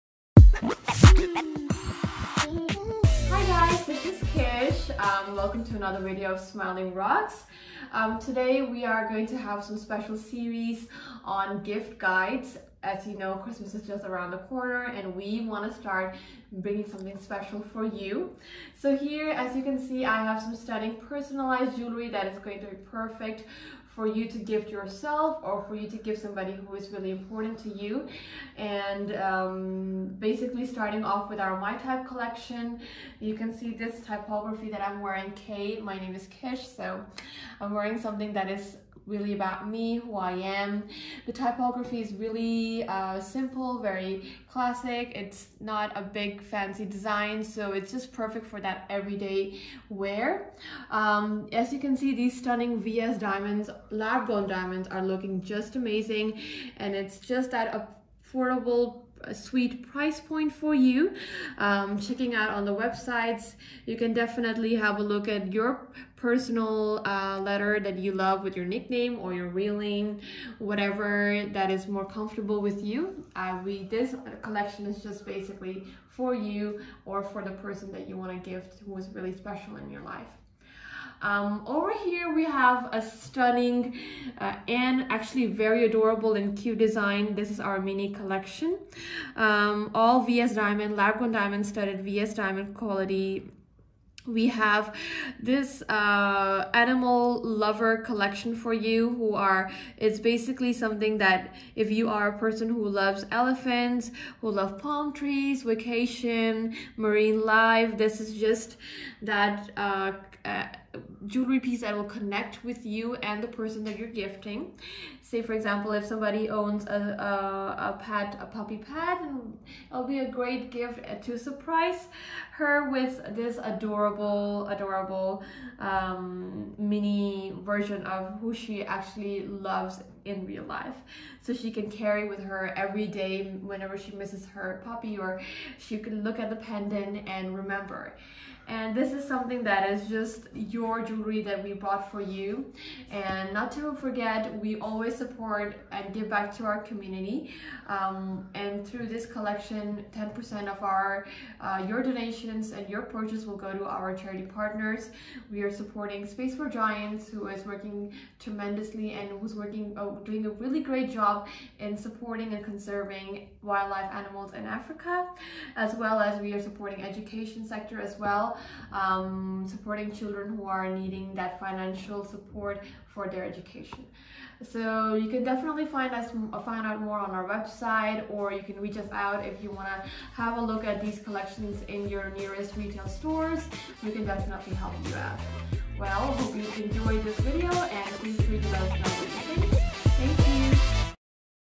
Holiday Gift Guide - My Type & mini Collections
Looking for timeless yet affordable gifts this holiday season?
Introducing My Type and mini Collections of our lab-grown diamond jewellry.

Get Timeless and Personalized gifts with our My Type collection.
TGift Yourself and your loved ones your favorite initials.

Our mini Collection is dedicated to protecting animals and bringing them into their natural inhabitance. You can find cute nautical designs of animals made in 10K WG and set with lab-grown diamonds.
With mini we are supporting organizations that do their best to protect these animals by donating 10% of each purchase to our charity partners.

All Smiling Rocks lab-grown diamond jewelry is accessible, effortless and empowering. Jewelry that we sure will make you smile